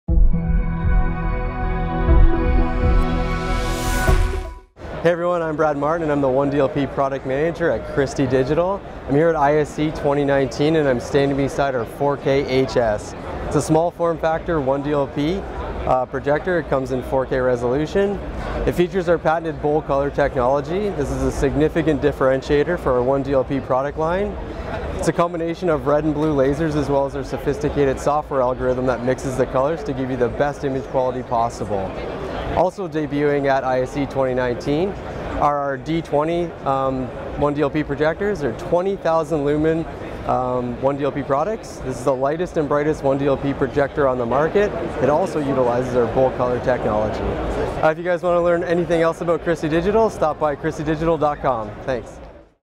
Christie launches small form factor 4K projector
Brad Martin reveals Christie Digital's small form factor 1DLP 4K projector - the 4KHS. It combines red and blue lasers as well as Christie's software algorithm.